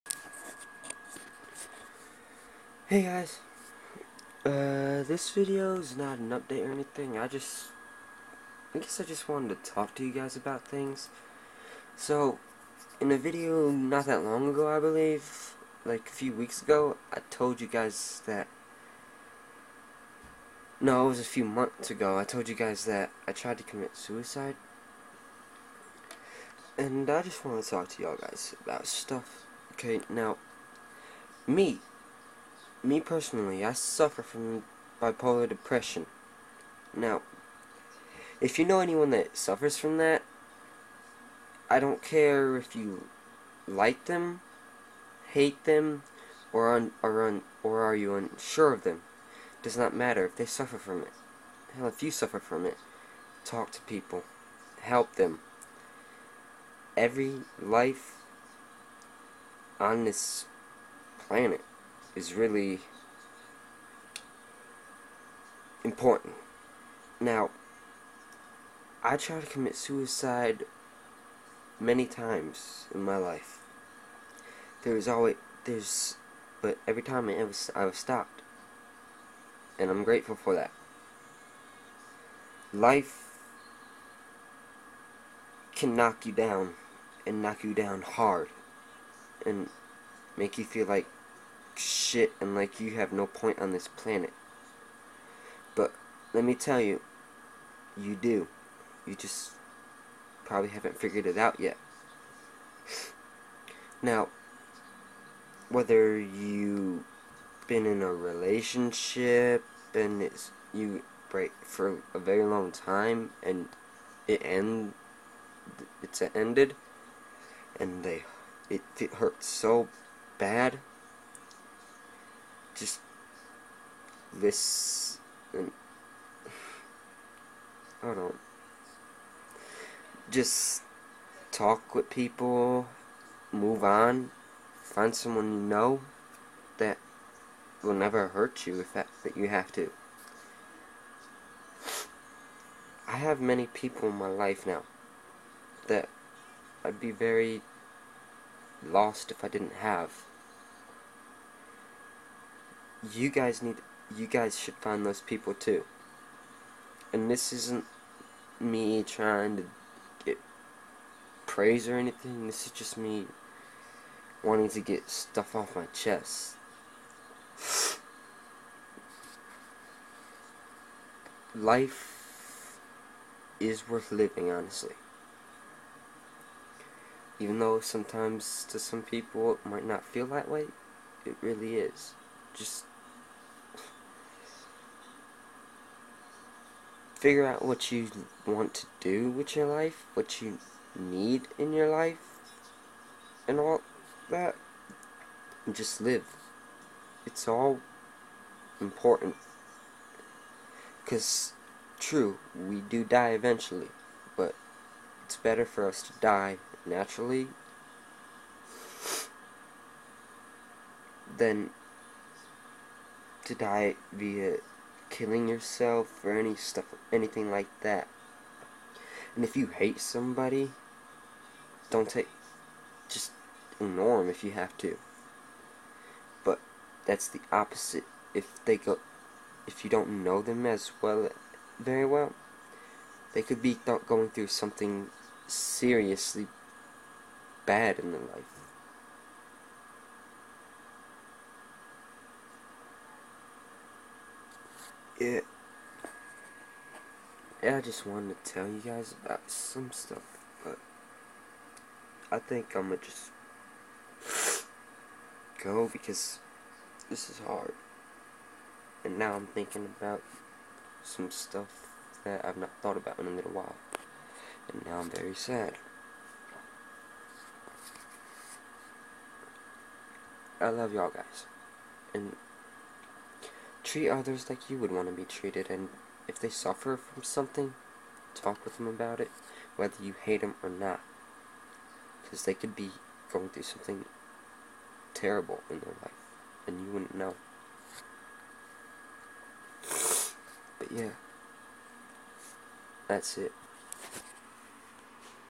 I Had To Talk About This (Sorry For Stuttring So Much)
I knew someone that committed suicide and I didn't help him because I didn't he was going through so much I failed to see it and I have live with that for the rest of my life but if guys talk to people that may suffer help them don't be aggresive about it tho but help them